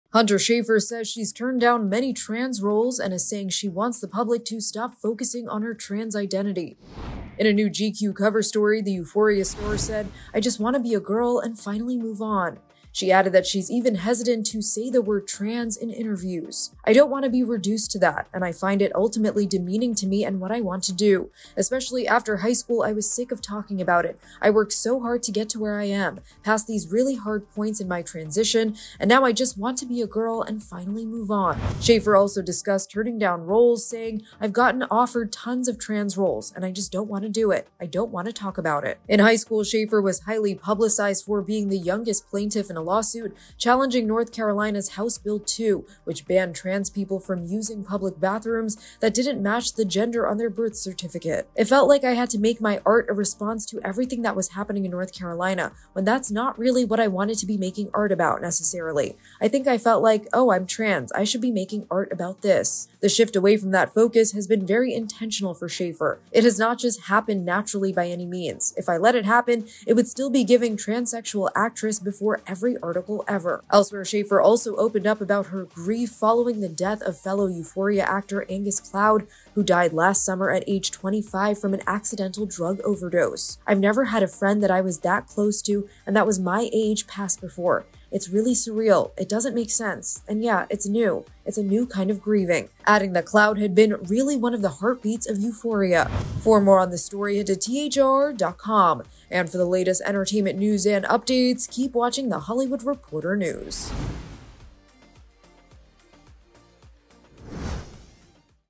'Euphoria' Star Hunter Schafer Says She's Turned Down "Tons" of Trans Roles | THR News
Hunter Schafer said she's turned down many trans roles and is saying she wants the public to stop focusing on her trans identity. In a new GQ cover story the 'Euphoria' star said, "I just want to be a girl and finally move on." She added that she's even hesitant to say the word "trans" in interviews.